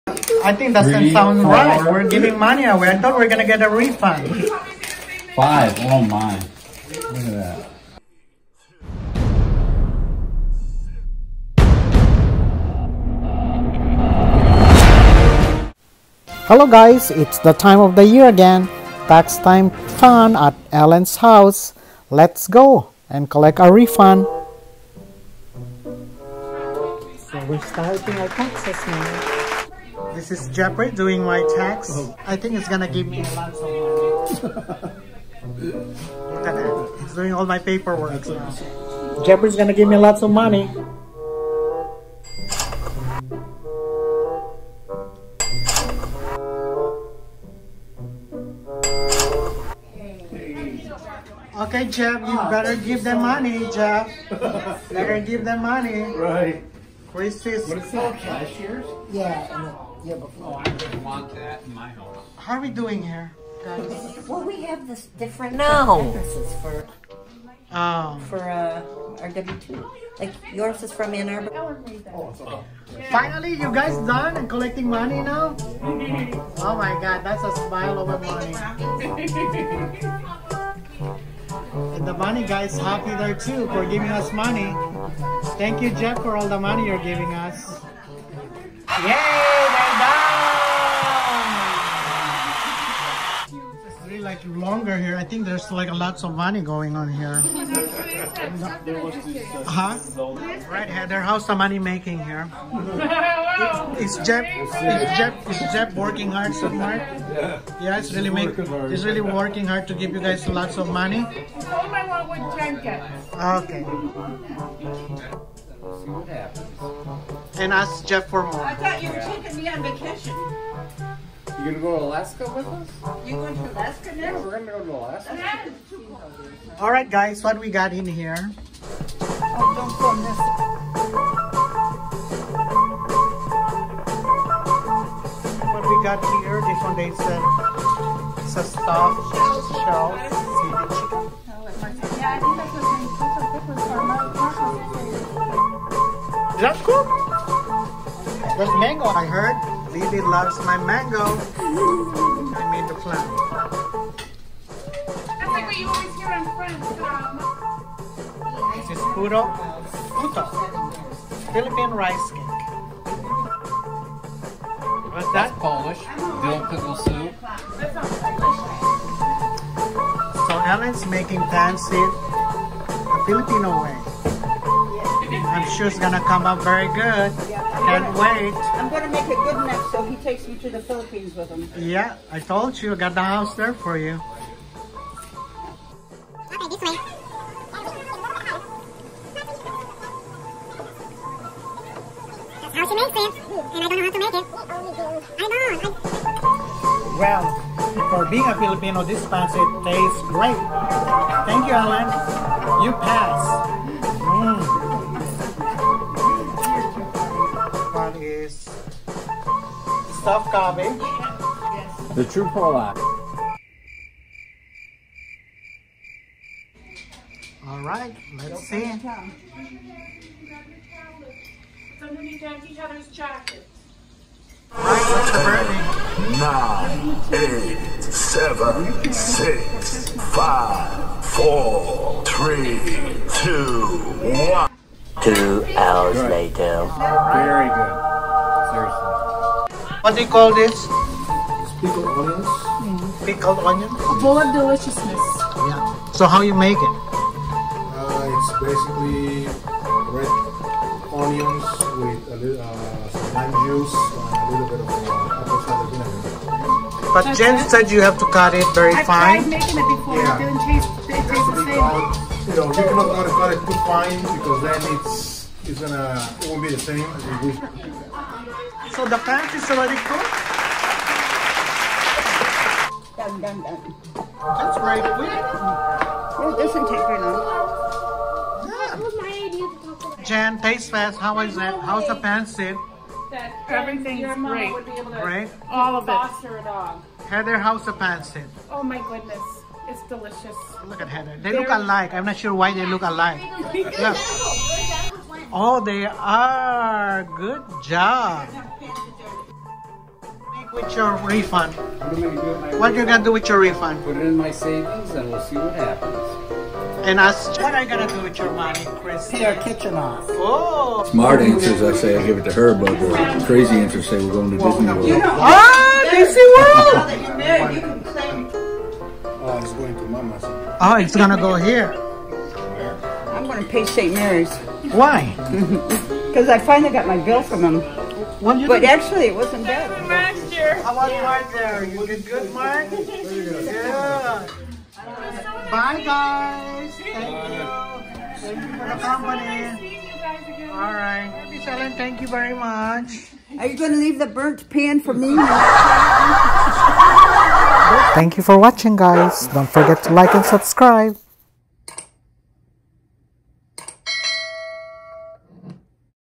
How to have fun while Tax Return filing
Hello guys this is ChuZoomZoom. Our yearly Tax Prep. Fun at our dear friend Ellen’s house. What a great host she is. Thanks Ellen and to our CPA Jeffrey Vedua. Everyone were Happy at the end of the night.